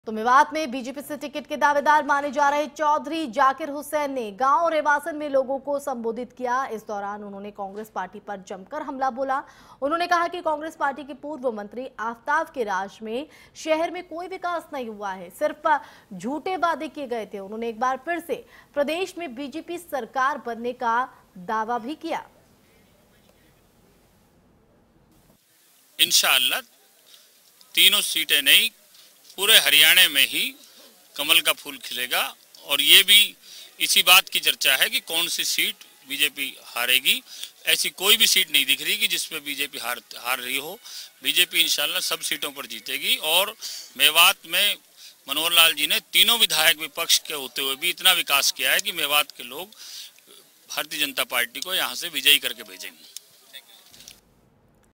जाकिर हुसैन का दावा...हरियाणा में फिर खिलेगा कमल
बीजेपी नेता ने जाकिर हुसैन ने दावा किया है कि हरियाण में फिर से बीजेपी की सरकार बनेगी..